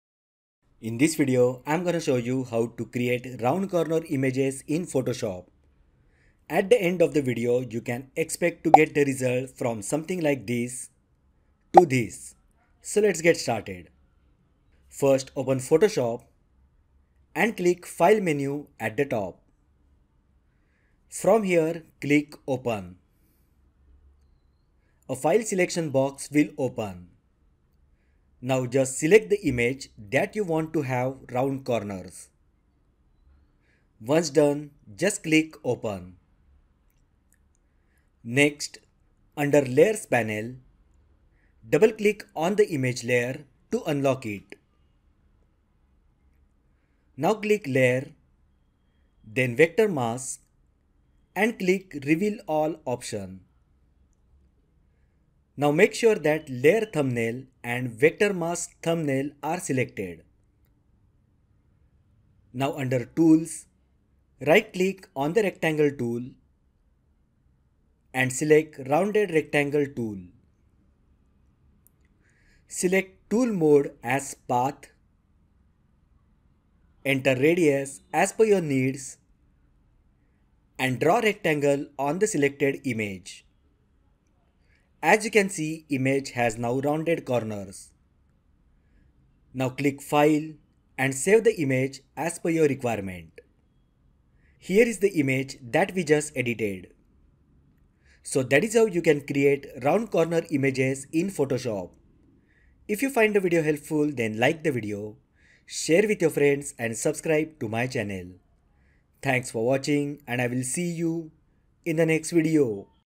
How to Create Round Corner Images in Photoshop
How to Create Round Corner Images in Photoshop. When it comes to designing images, small details can make a big difference. One such detail that can enhance the look of your pictures is adding rounded corners. Photoshop, a popular graphic design tool, offers a straightforward way to achieve this effect. By following a few simple steps, you can give your images a modern and polished appearance with rounded corners. Let's explore how to do it:

 - Open Photoshop: Begin by launching Photoshop on your computer. Look for the "File" menu at the top of the screen.

 - Open Image: Click on "File" and then select "Open" from the dropdown menu. This action will prompt a window to appear, allowing you to navigate to the image file you wish to edit. Once you've located the image, click "Open" to import it into Photoshop.

 - Unlock Image Layer: In Photoshop, images are typically placed on layers, which can be locked to prevent unintended changes. To modify the image freely, you'll need to unlock its layer. Locate the Layers panel, usually found on the right side of the Photoshop interface. Double-click on the image layer to unlock it.

 - Apply Vector Mask: With the image layer unlocked, it's time to apply a vector mask. This mask will define the area where the rounded corners will be added. Go to the "Layer" menu, then "Vector Mask," and choose "Reveal All" to create the mask.

 - Select Rounded Rectangle Tool: Photoshop provides various shape tools for creating geometric shapes. For rounded corners, we'll use the Rounded Rectangle Tool. Find the Rectangle Tool in the Tools menu, right-click on it, and select "Rounded Rectangle Tool."

 - Adjust Settings: Before drawing the rounded rectangle, adjust the tool settings according to your preferences. You can specify the radius of the rounded corners based on how subtle or pronounced you want them to be.

By following these steps, you can easily create round corner images in Photoshop, adding a touch of elegance to your designs. Whether you're working on personal projects or professional endeavors, this technique can elevate the visual appeal of your images with minimal effort.